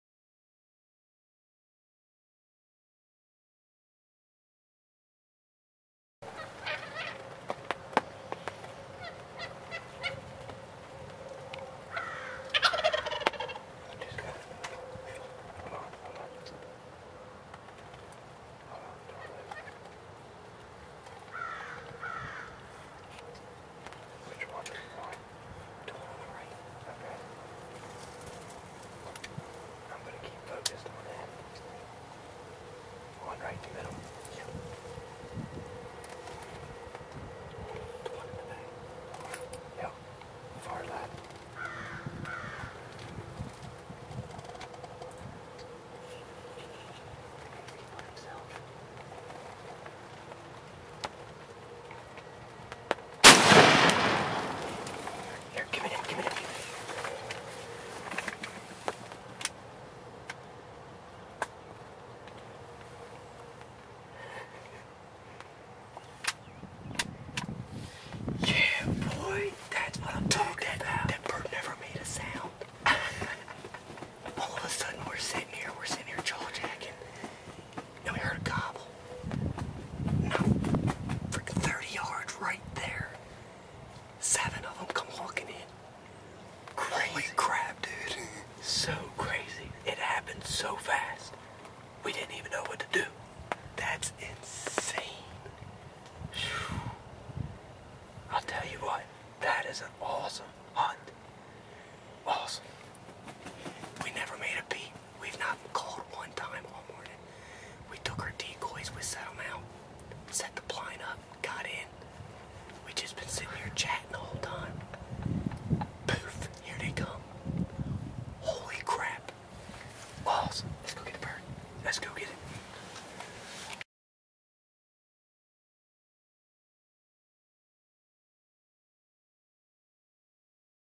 Delaware Wild Turkey Hunting V7
2 hunters from Delaware bring in a couple of strutting Toms in along with their hens. They pull off the shot and bring home a nice bird.

What a hunt folks!